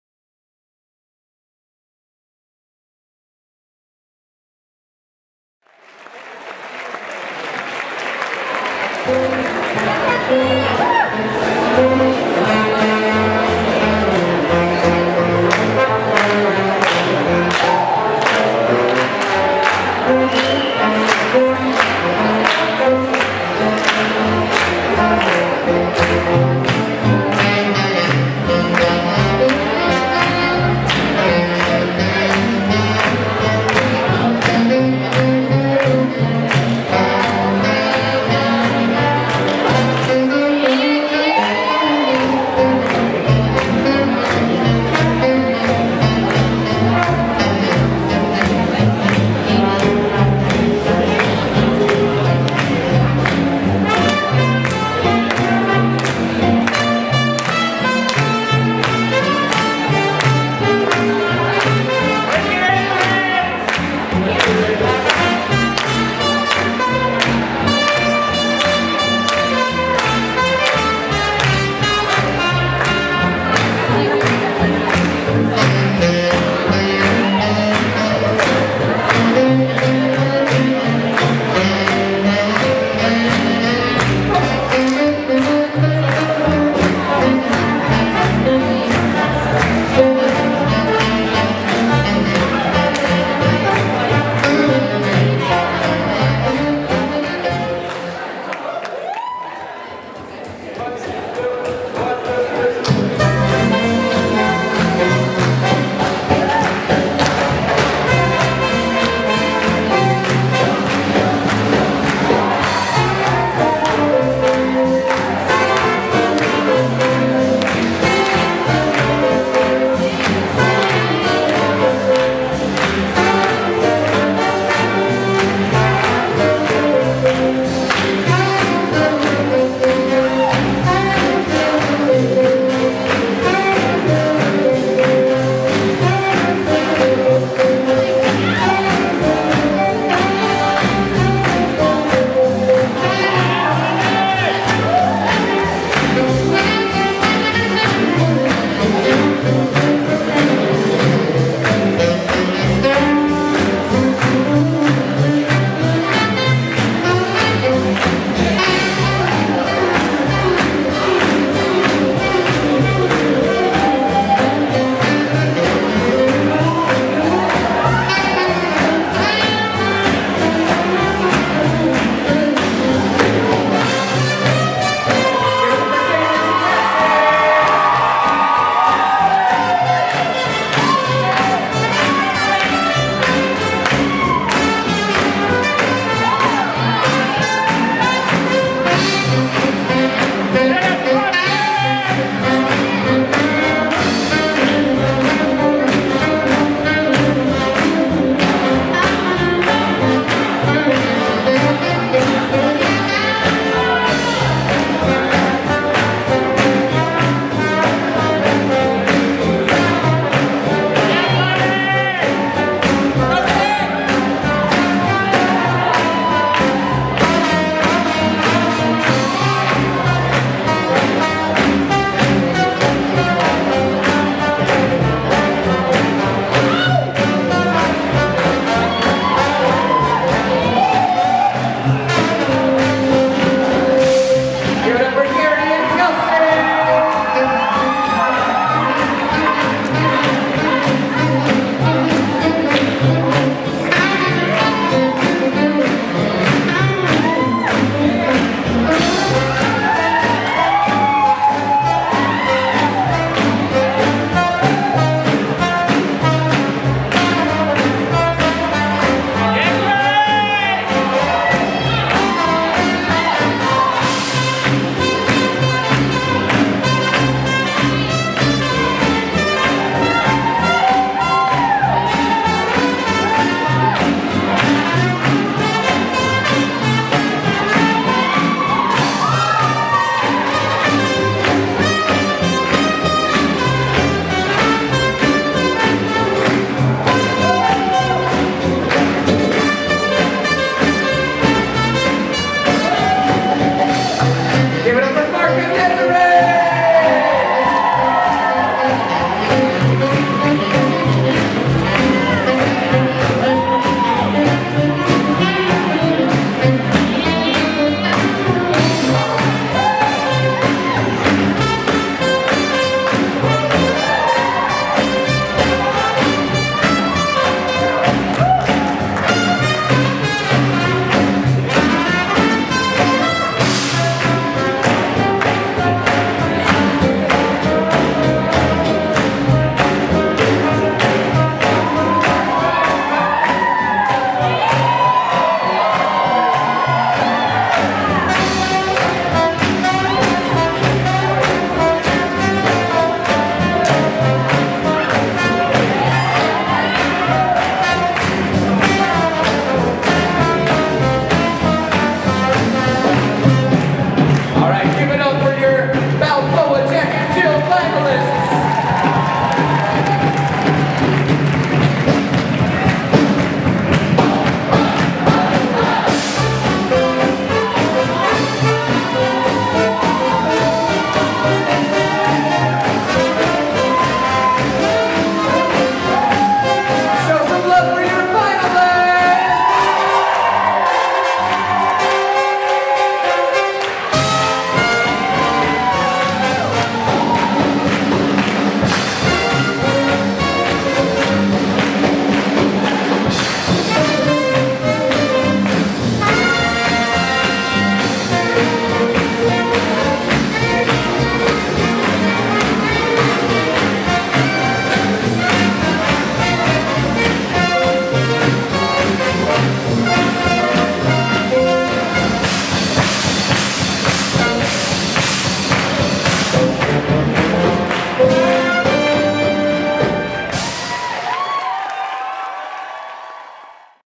Hawkeye Swing Fest 2015 - Balboa J&J Finals
Placing:
1st: Mark Calkins & Desiree Roffers
2nd: Gary Chi & Kelsey Mickelson
3rd: David Plotkin & Cassie Stoa